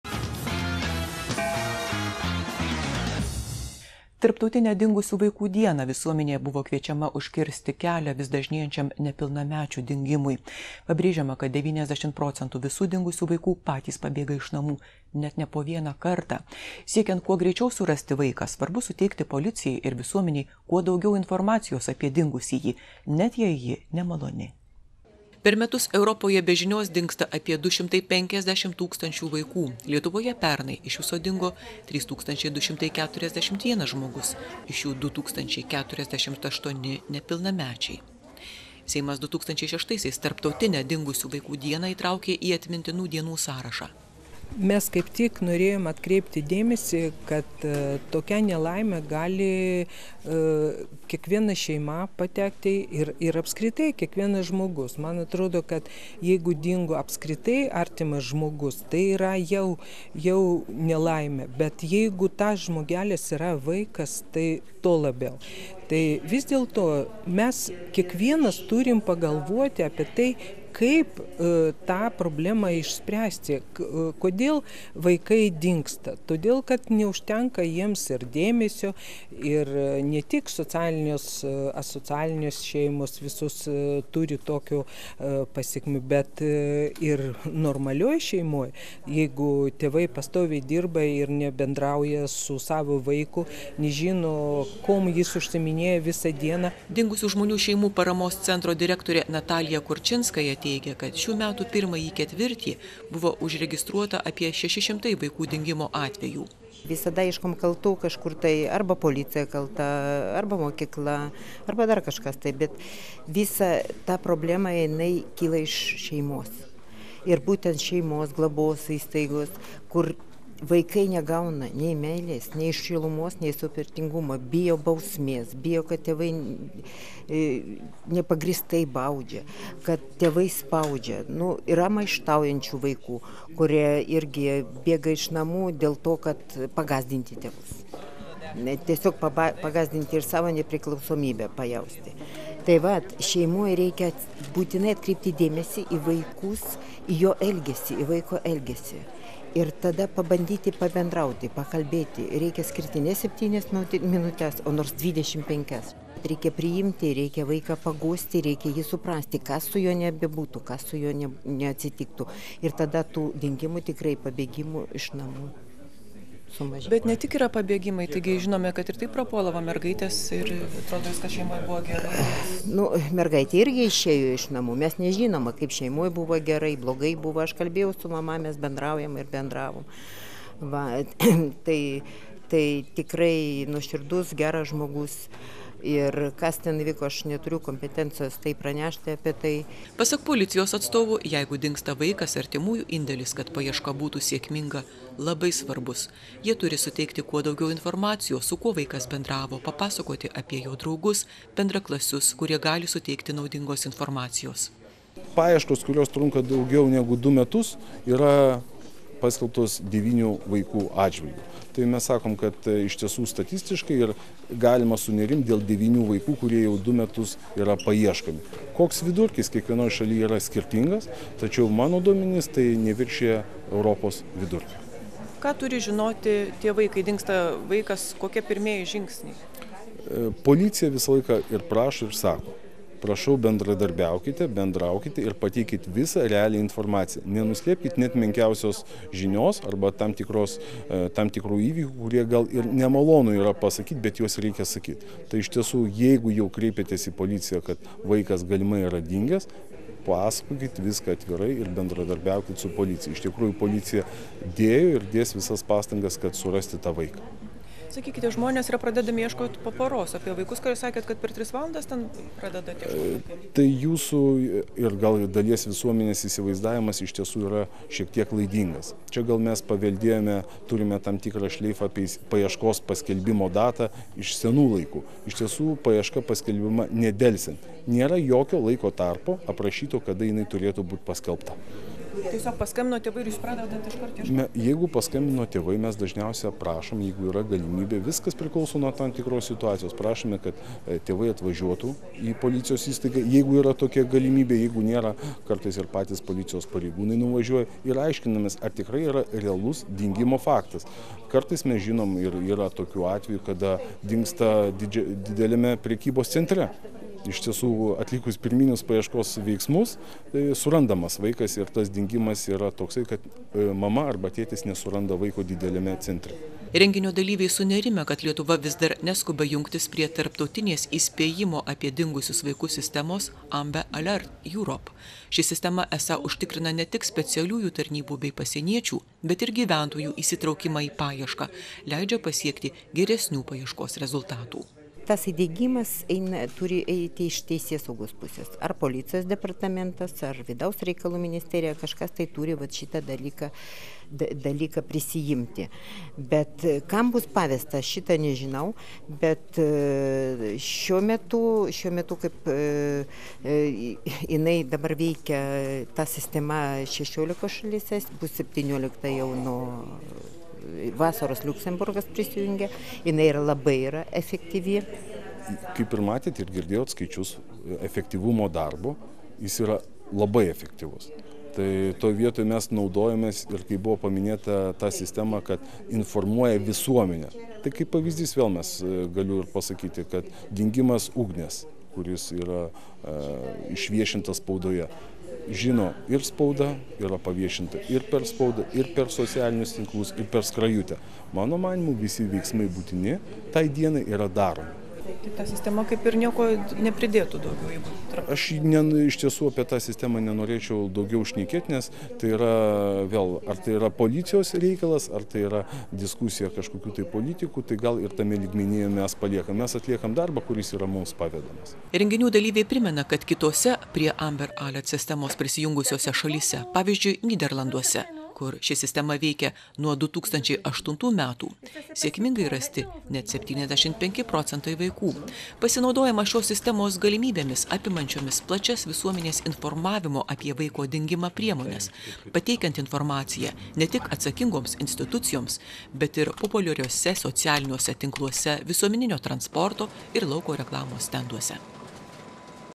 Seime paminėta tarptautinė dingusių vaikų diena
2016 m. gegužės 25 d. įvykis. Tarptautinę  dingusių vaikų dieną  visuomenė buvo kviečiama užkirsti kelią vis dažnėjančiam nepilnamečių dingimui.